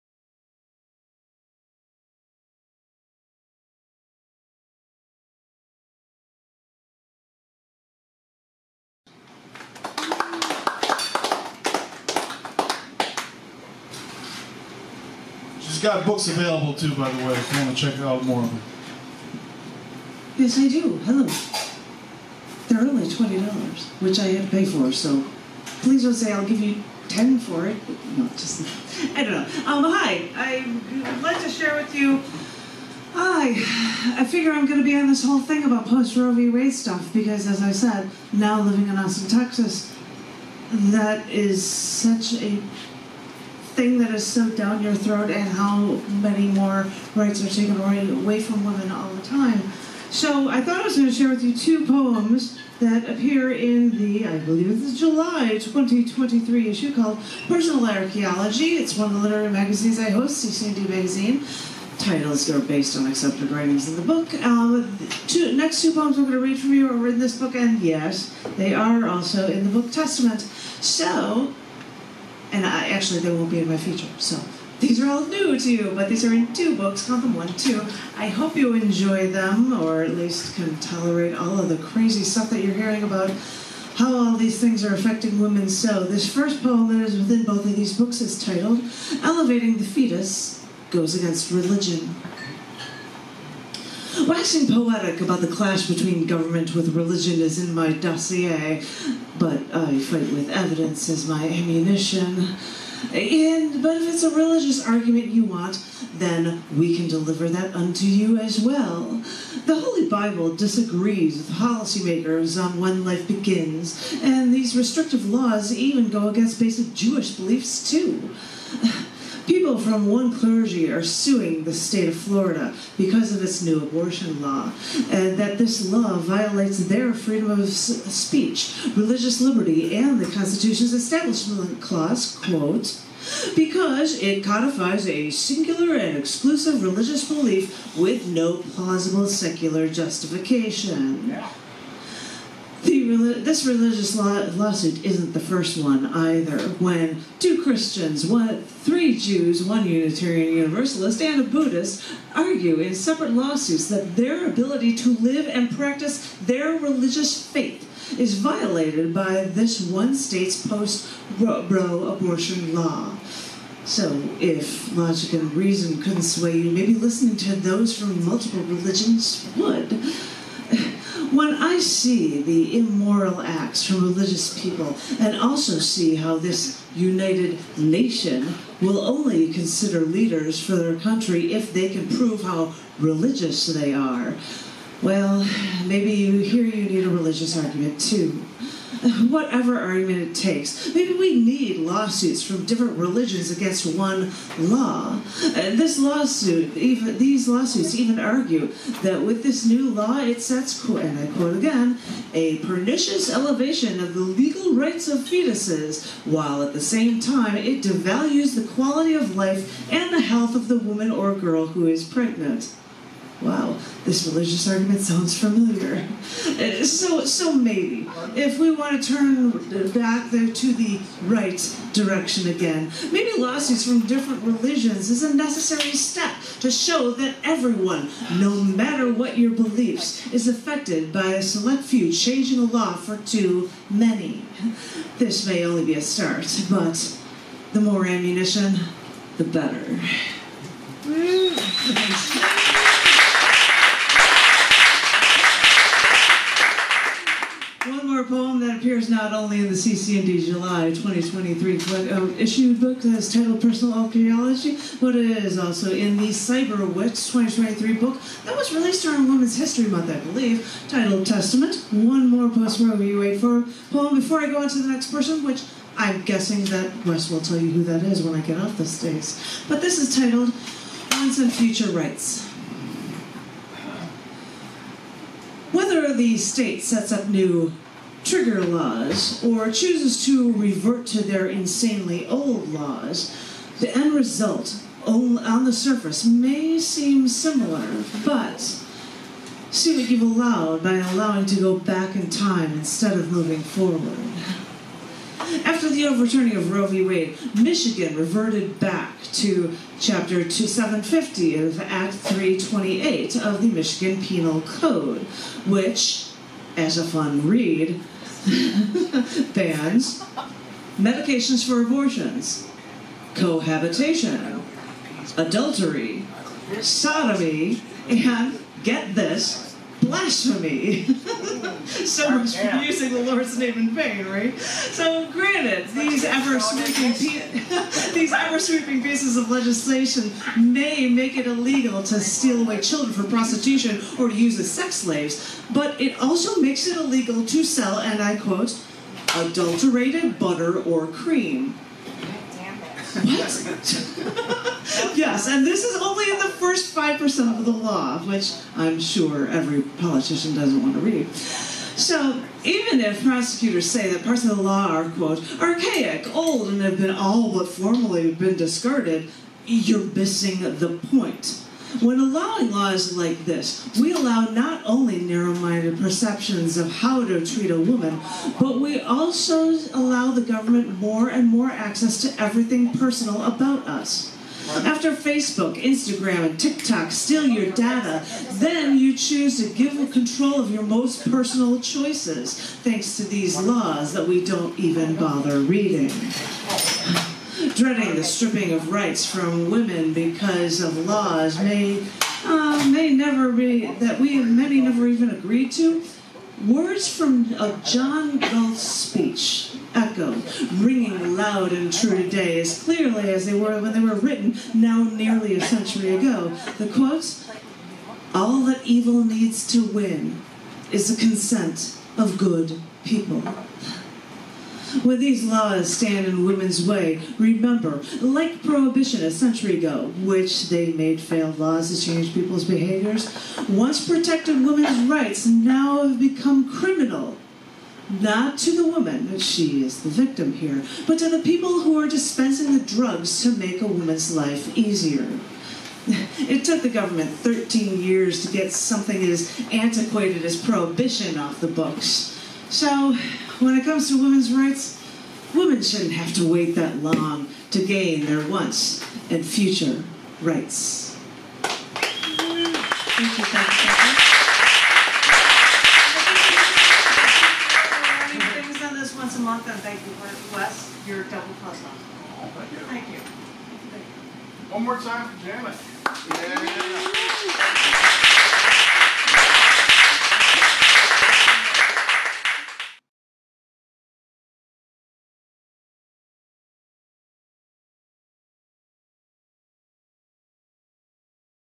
Janet Kuypers’ 2nd “Testament” poetry performance @ Chicago’s The Gallery Cabaret on 8/9/23 PL2500.
Live at the “Poetry Open Mic at The Gallery Cabaret with feature Janet Kuypers” in Chicago on August 8th, 2023 (or 8/9/23, or 20230809, or 9 Aug 2023), Janet Kuypers (who used to run an open mic at this venue when she lived in Chicago, came to the stage and started the evening by reading her poems “Elevating the Fetus Goes Against Religion” and “Once and Future Rights” from the v335 7/23 cc&d issue/book “Personal Archaeology” & the CyberWit 2023 Janet Kuypers book “Testament” to a live audience.

This was filmed with a Panasonic Lumix T56 camera, a Panasonic Lumix 2500 camera, and a Samsung S7 camera (the Samsung S9 camera was also released zoomed), and it was also live video streamed from a Samsung S9 camera on Facebook.